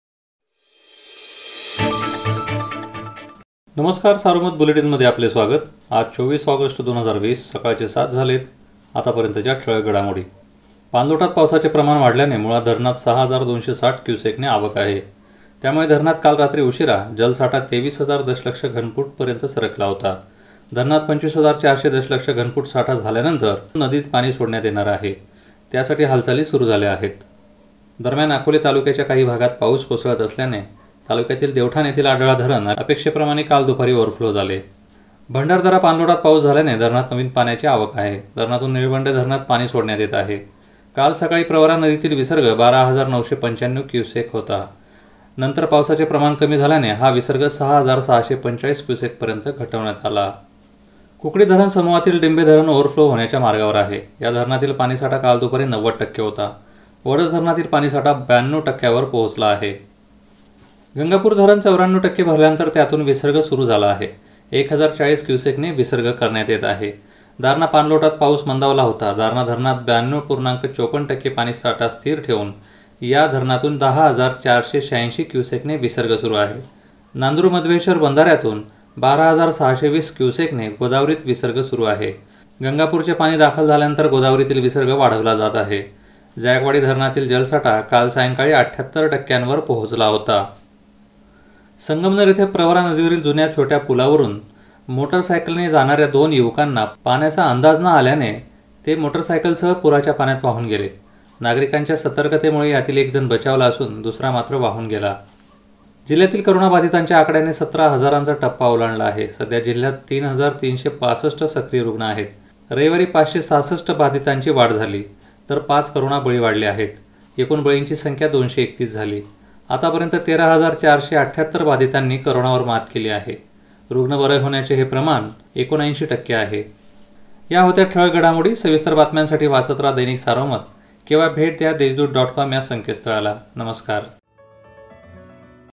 मुळा धरण दोन दिवसात तुडूंब? | आढळा ओव्हरफ्लो | जायकवाडी@78% | ahmednagar | shrirampur
-पाणलोटात  पावसाचे प्रमाण वाढल्याने मुळा धरणात 6 हजार 260 क्युसेकने आवक आहे. त्यामुळे धरणात काल रात्री उशीरा जलसाठा 23 हजार दसलक्ष घनफूटपर्यंत सरकला.

_अकोले तालुक्याच्या काही भागात पाऊस कोसळत असल्याने तालुक्यातील देवठाण येथील आढळा धरण अपेक्षेप्रमाणे काल दुपारी ओव्हरफ्लो झाले.

_भंडारदरा पाणलोटात पाऊस झाल्याने धरणात नवीन पाण्याची आवक झाली. धरणातून निळवंडे धरणात पाणी सोडण्यात येत आहे.

_कुकुडी धरण समुहातील डिंभे धरण ओव्हरफ्लो होण्याच्या मार्गावर आहे. या धरणातील पाणीसाठा काल दुपारी 90 टक्के होता.  वडज धरणातील पाणीसाठा 92 टक्के होता.

_गंगापूर धरण 94 टक्के भरल्यानंतर त्यातून विसर्ग सुरू झाला आहे. 1 हजार 40 क्युसेकने विसर्ग आहे.
_दारणा पाणलोटात पाऊस मंदावला होता. दारणा धरणात 92 पूर्णांक 54 टक्के पाणीसाठा स्थिर ठेवुन या 10 हजार 486 क्युसेकने विसर्ग सुरु आहे.
_जायकवाडी 78 धरणातील जलसाठा काल सायंकाळी 78 टक्क्यांवर पोहचला होता.